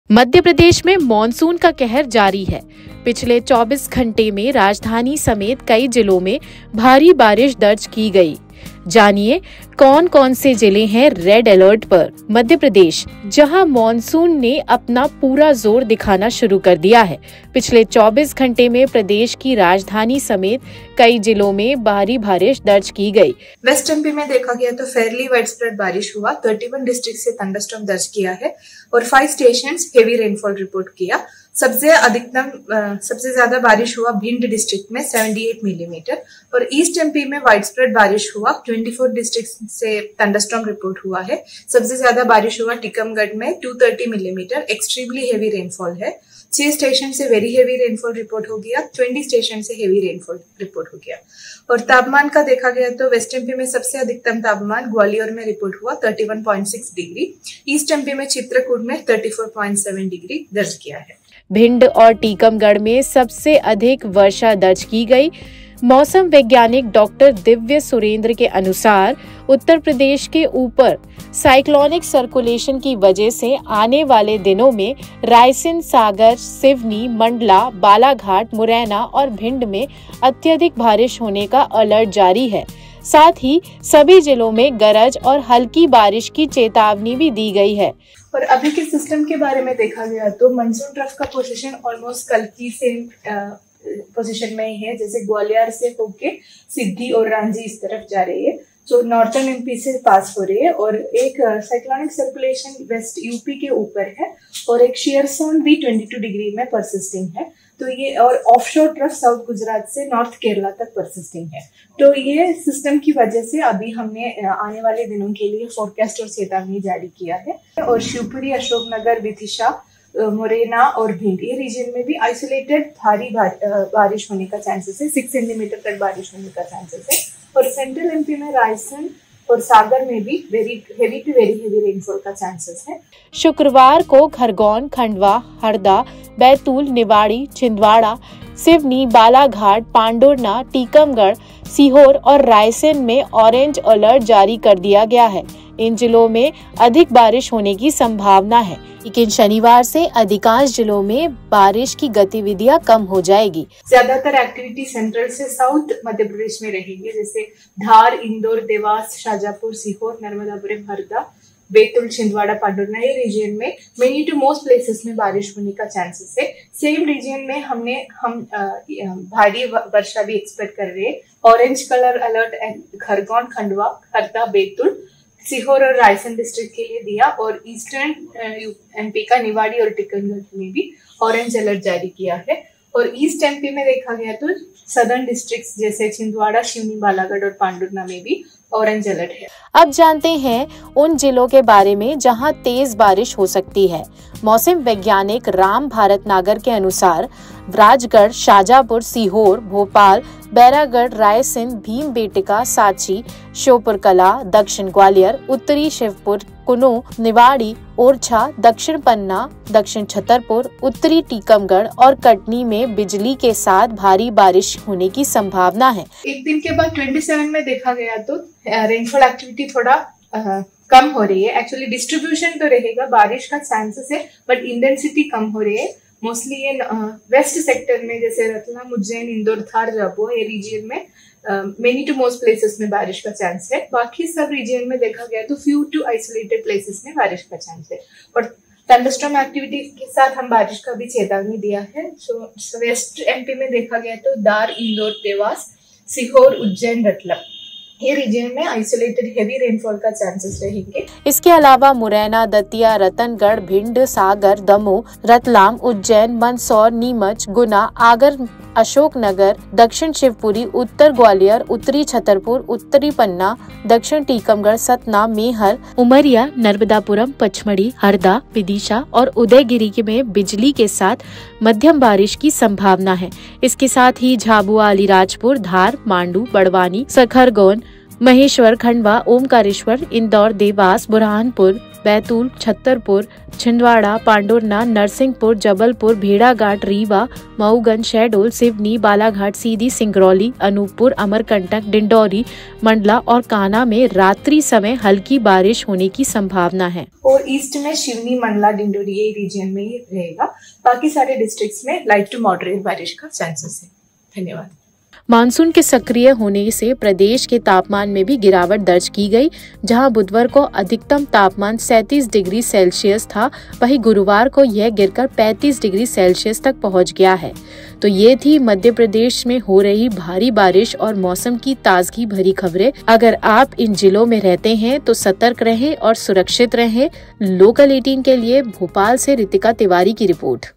Weather Update: मॉनसून की मार, मध्य प्रदेश के इन जिलों में भारी बारिश का अलर्ट!
मध्य प्रदेश में मॉनसून का कहर जारी है. पिछले 24 घंटों में राजधानी समेत कई जिलों में भारी बारिश दर्ज की गई है। जानिए कौन-कौन से जिले हैं रेड अलर्ट पर और कहाँ हो सकती है अत्यधिक बारिश!  मध्य प्रदेश, जहां मॉनसून ने अपना पूरा जोर दिखाना शुरू कर दिया है। पिछले 24 घंटों में प्रदेश की राजधानी समेत कई जिलों में भारी बारिश दर्ज की गई है।

Monsoon continues to wreak havoc in Madhya Pradesh. In the last 24 hours, heavy rain has been recorded in many districts including the capital. Know which districts are on red alert and where there may be excessive rain!  Madhya Pradesh, where monsoon has started showing its full force. In the last 24 hours, heavy rain has been recorded in many districts including the state capital.